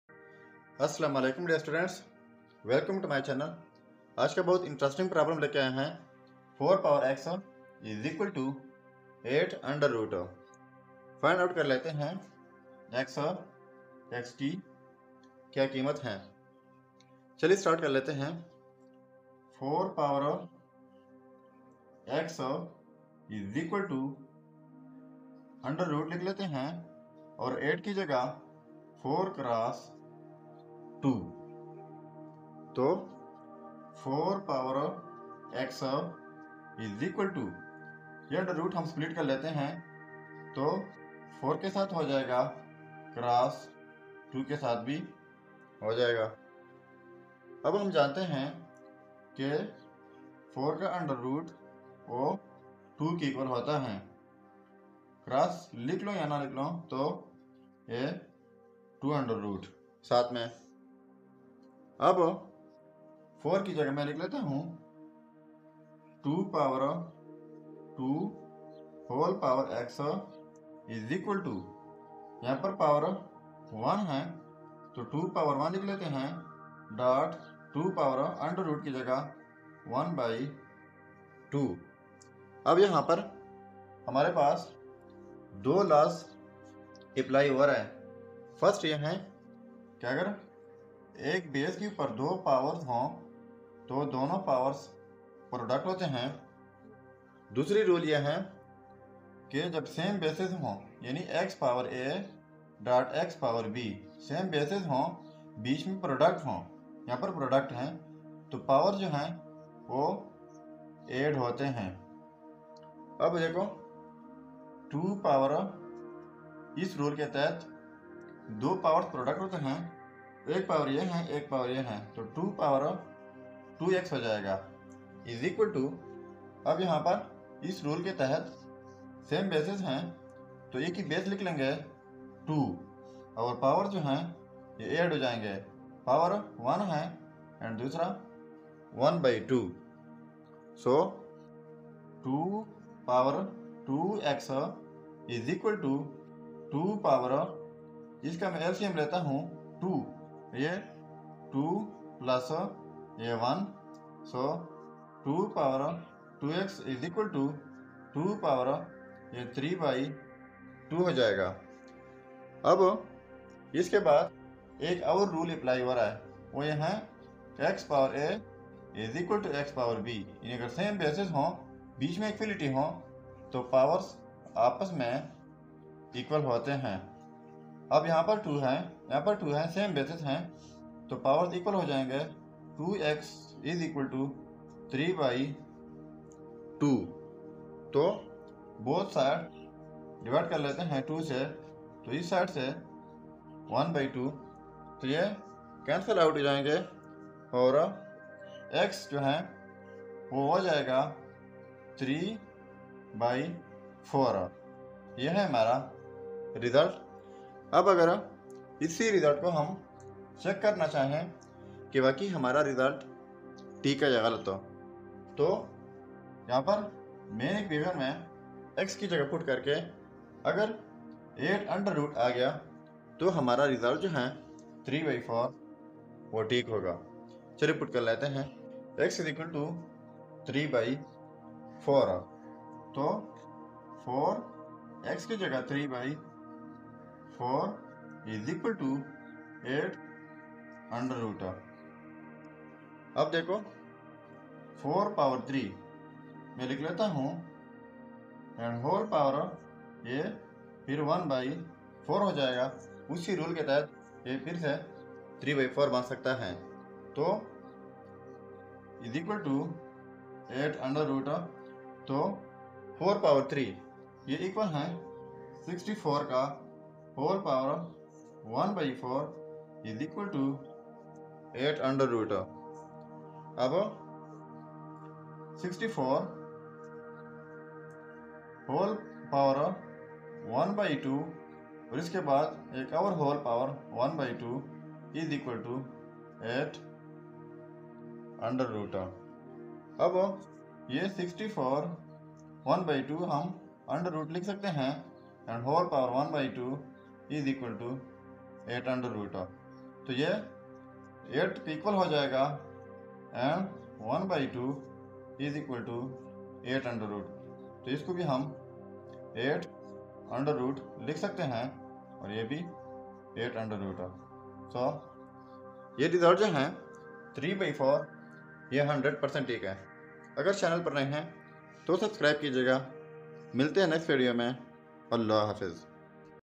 Math Olympiad 4ˣ = √8 | A Nice Olympiad Exponential Problem | Algebra
Join us on a mathematical journey as we tackle a fascinating Olympiad exponential problem in this YouTube video. By exploring the equation 4ˣ = √8,  viewers will have the opportunity to sharpen their algebraic skills and deepen their understanding of exponential functions. This video is a must-watch for anyone passionate about mathematics or seeking to enhance their problem-solving abilities in the realm of algebra.

❤️Now You Can Follow❤️